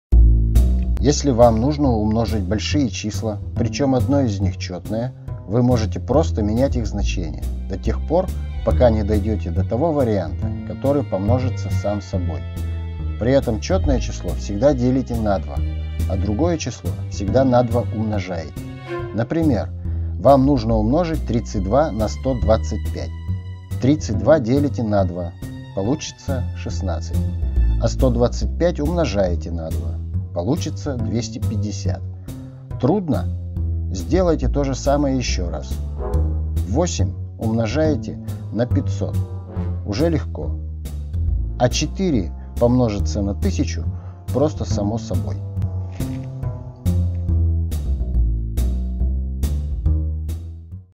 Умножение больших чисел в уме
Если вам нужно умножать большие числа, причем одно из них — четное, вы можете просто менять их значение до тех пор, пока не дойдете до того варианта, который помножится сам собой. При этом четной число всегда делите на 2, а другое число всегда на два умножайте.